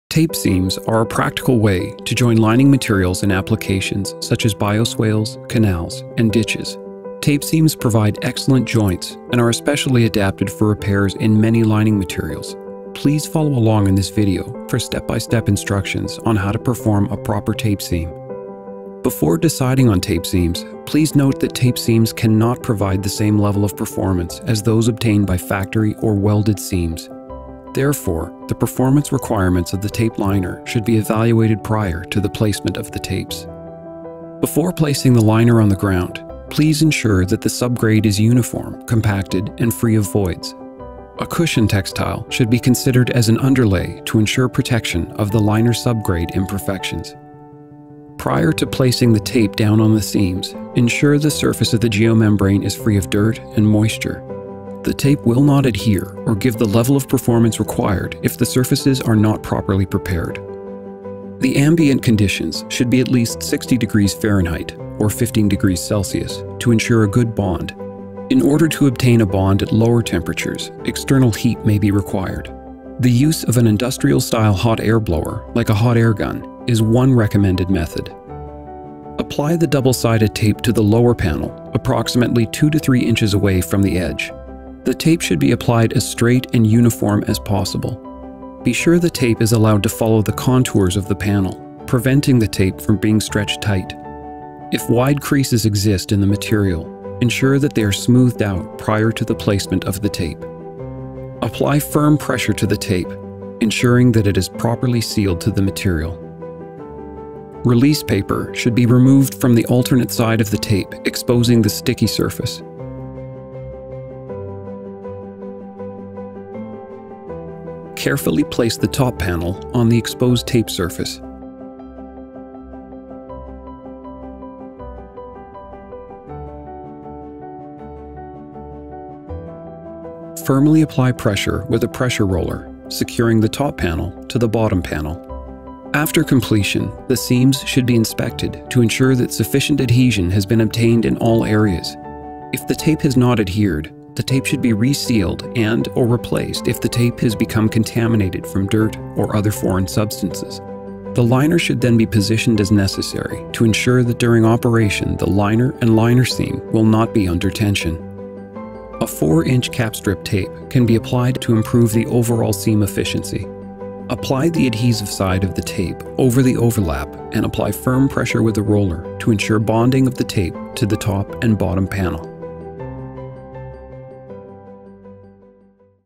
Tape Seaming of Geomembrane Panels
Tape seams are a practical way to join lining materials in applications such as bioswales, canal liners, and in the seaming of geomembrane panels when welding equipment is not available. Tape seams provide excellent joints and are especially adapted for repairs in many lining materials. Please follow along in this video for step-by-step instructions on how to perform a proper tape seam.